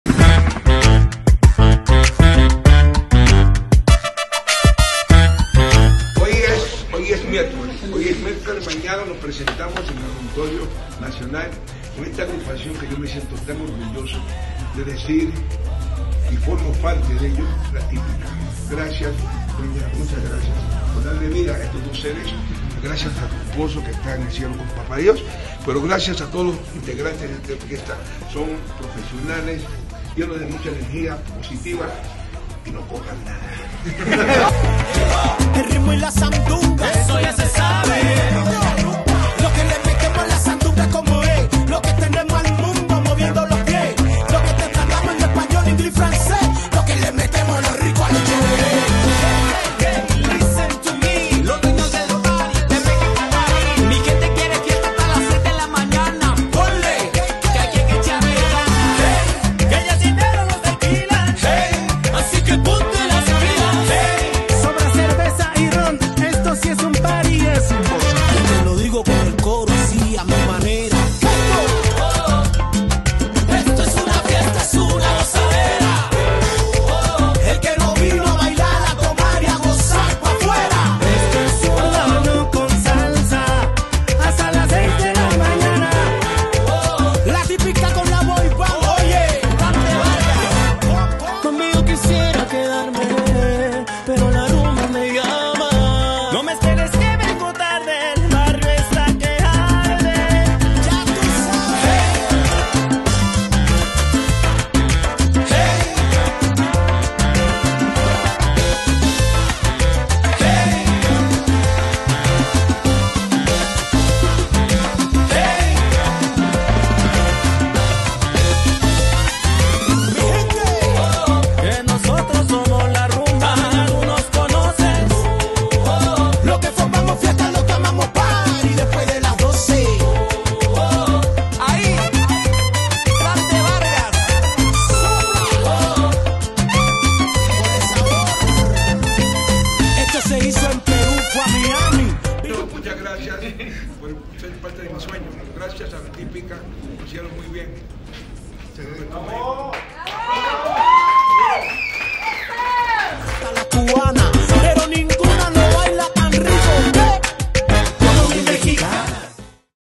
Internacional Orquesta La Típica y Tito Nieves detrás de cámaras | Auditorio Nacional 2023
Contrataciones:
Enrique Cervantes | Ignacio Vilchis